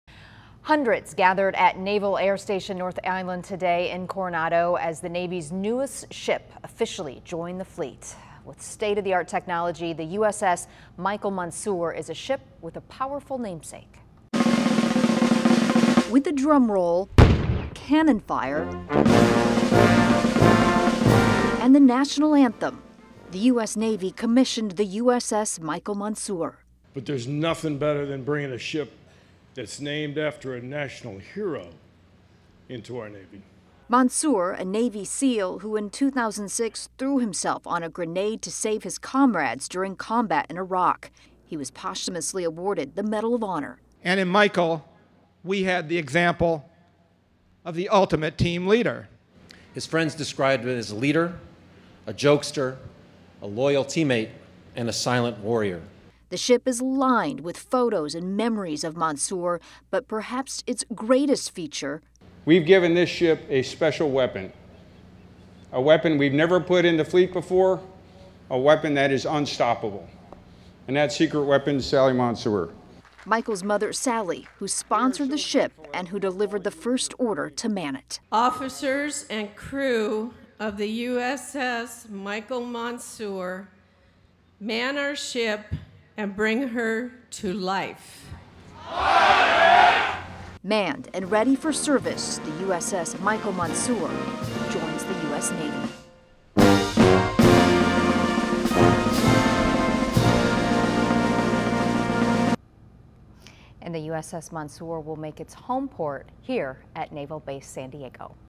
Navy commissions USS Michael Monsoor
The U.S. Navy commissioned the USS Michael Monsoor Saturday.